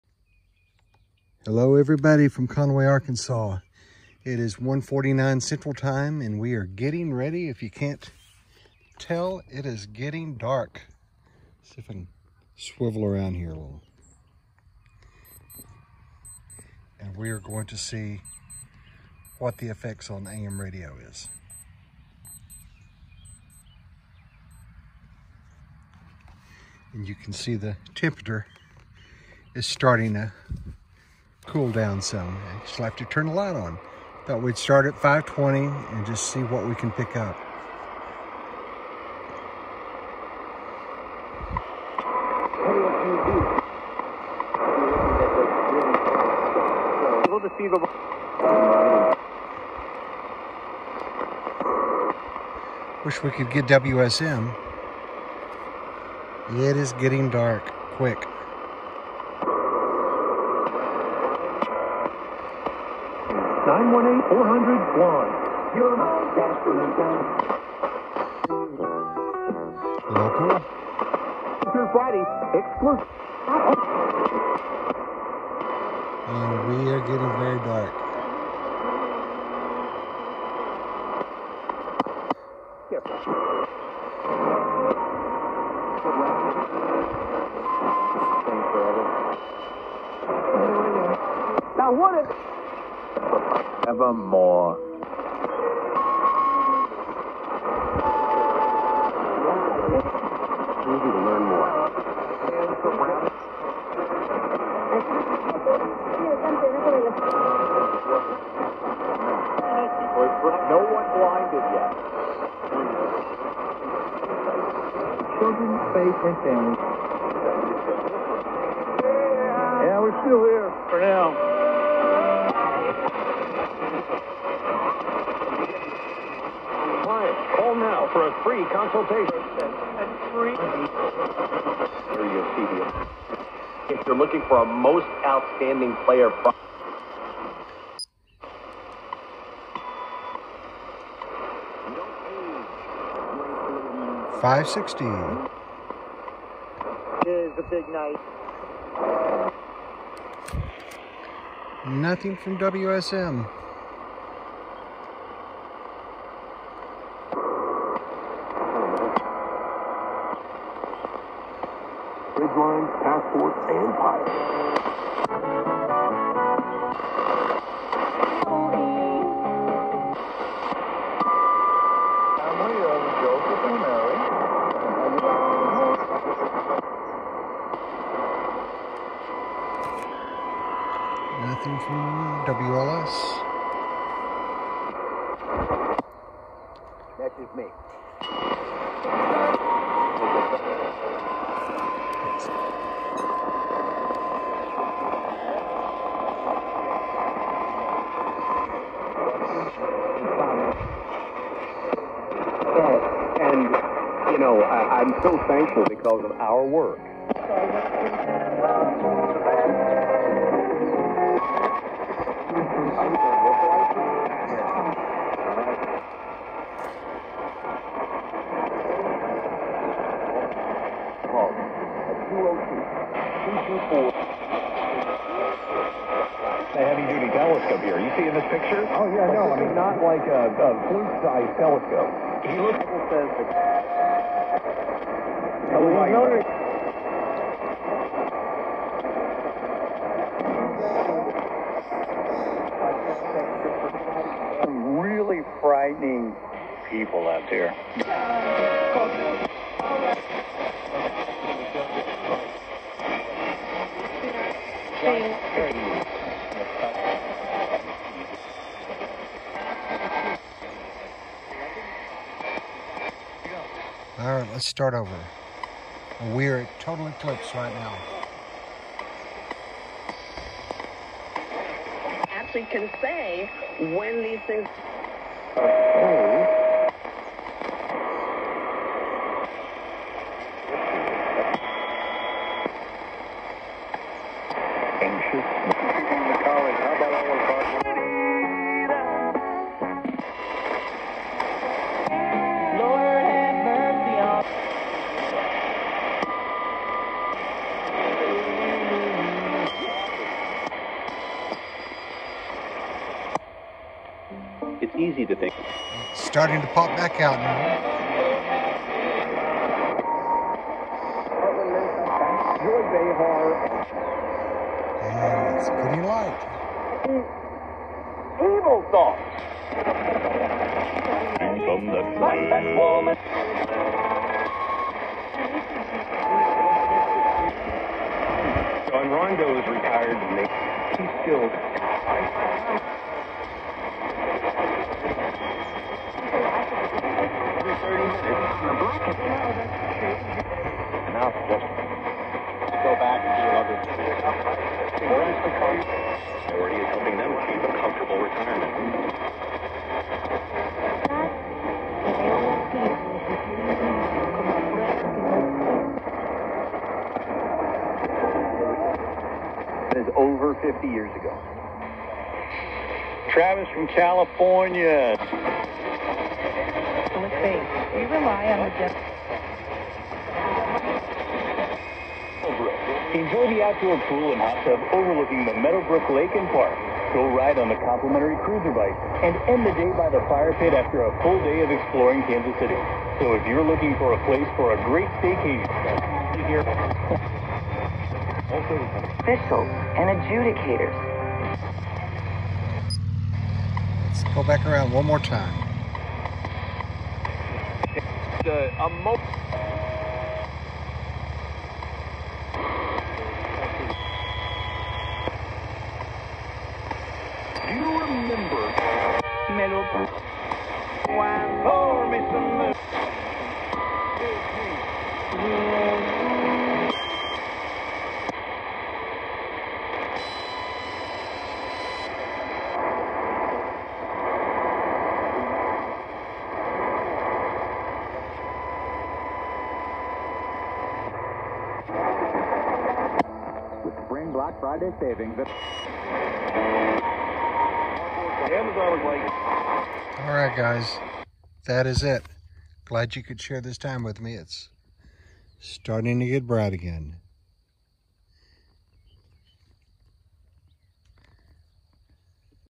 Listening to AM radio in Arkansas during the 2024 Eclipse
Observing the AM radio band conditions in Arkansas during the Eclipse 2024.